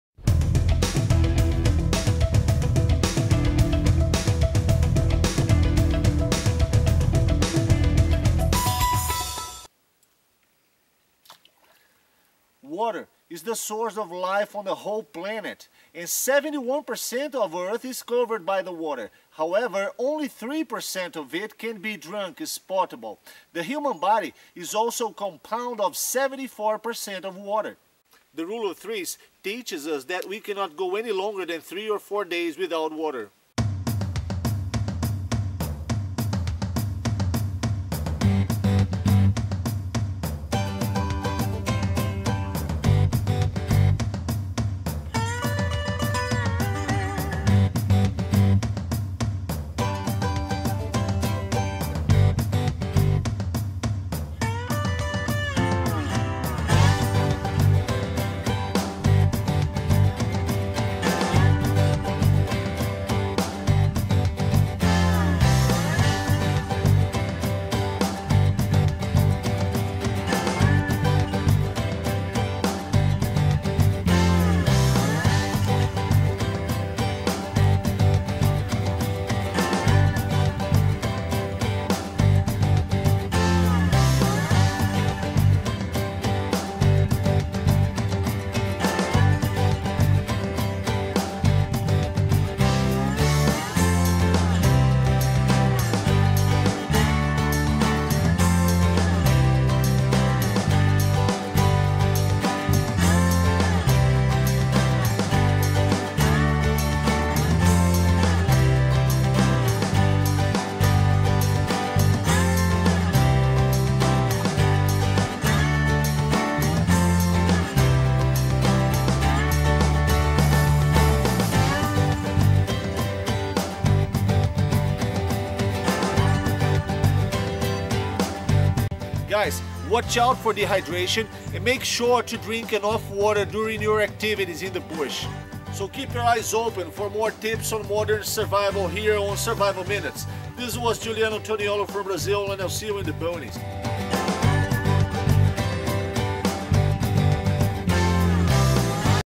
Survival Minutes 05: Hydration
Water is life, specially when stranded in the wilderness. Here we discuss the importance of keeping hydration as well as some ways to collect, filter and treat your water.